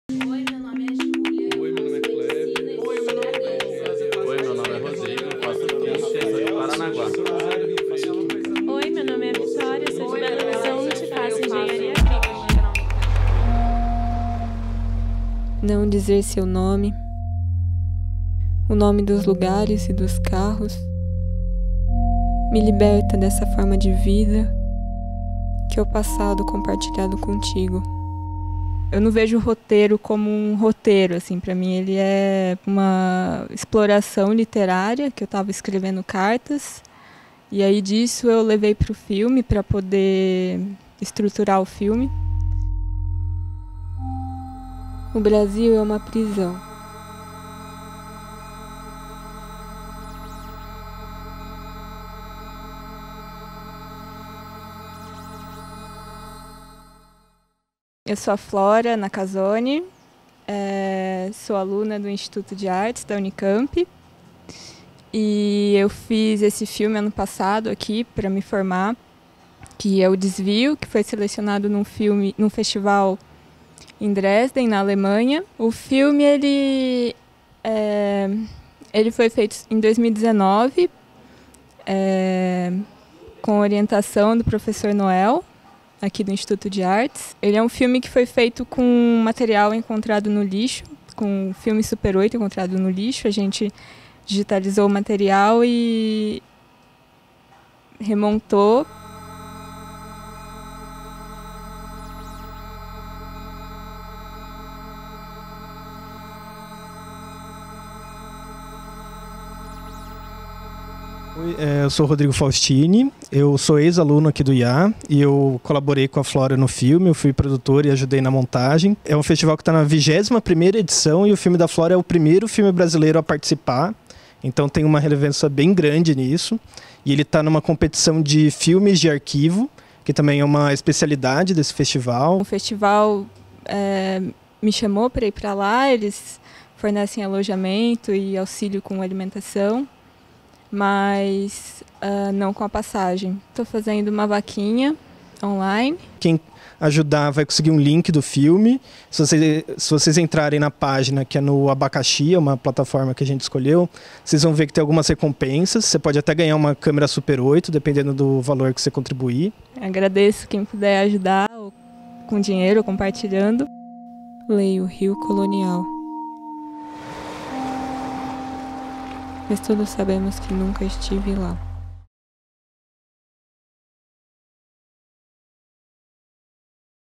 Curta-metragem de aluna da Unicamp é selecionado para festival na Alemanha
O filme "Desvio", realizado pela aluna Flora Nakazone, do Instituto de Artes (IA) da Unicamp, foi selecionado para estrear mundialmente no 21º Dresdner Schmalfilmtage, na Alemanha, em 19 de março. Confira os depoimentos da diretora e do produtor do curta.